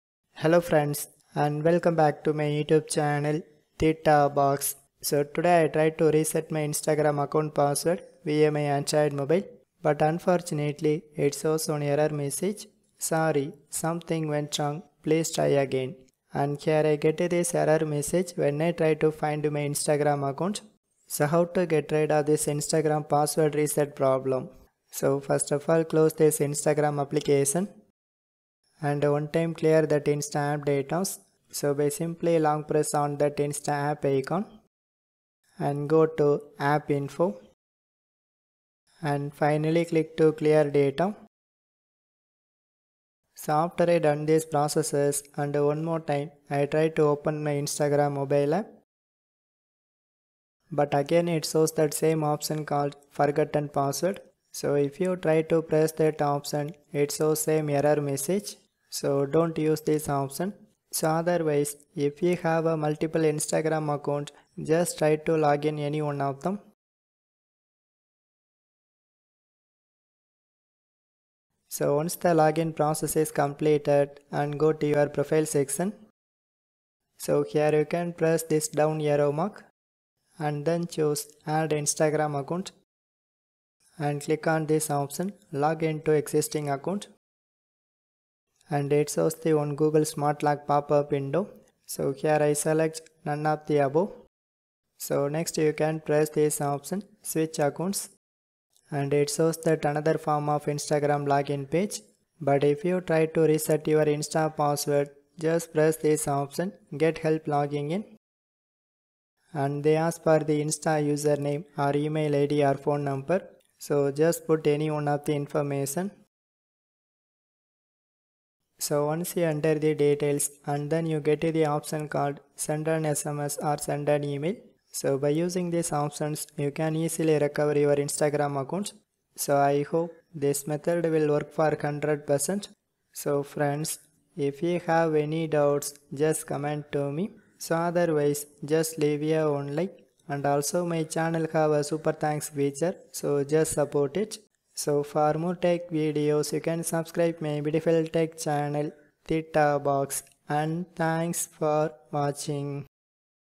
Fix instagram password reset problem, sorry something went wrong please try again problem solved
how to fix instagram password reset problem, sorry something went wrong please try again problem solved.

Here i using the 2023 released insta app,but in future 2024 if they release any new update some setting location may be varrying.

This video has english subtitle at same time you can translate to kaise kare hindi,tamil,bangla telugu,kannada,sinhala and malayalam languages.

Today i get the following one error notification while i try to change my instagram account password :

Find your account

Can't reset your password?.

Sorry,something went wrong

OK

How to solve instagram sorry something went wrong please try again later :

1.One of my subscriber ask to me,why it shows this kind of message and what is the meaning of this issue?.

2.Sometime this is occurs due to instagram server down problem.

3.So first of all check your insta server status and also check your internet connection like wifi and mobile data network.

4.So anyway if you like to fix this issue,just clear that insta app data's.

5.And also login your another instagram account.

6.Now you get the another form of instagram login page,here you can press this link "Get help logging in".

7.And then put your username or email or phone number to reset your password.

8.So i hope if you follow this simple steps,finally this problem is solved in permanently.